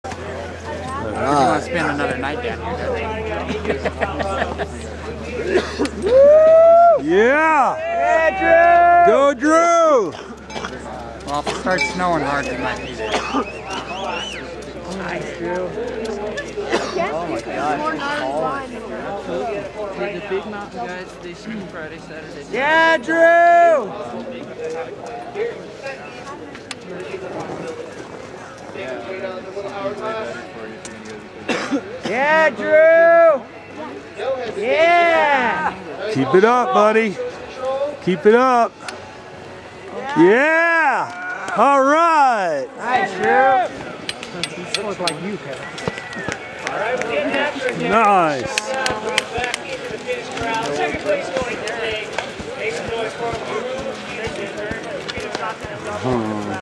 Crested Butte Extremes Final Day Drew Petersen 2010
This was my final run at the U.S. Extreme Freeskiing Championships in Crested Butte, Colorado.  I finished 2nd in the Junior Men 15-17 division.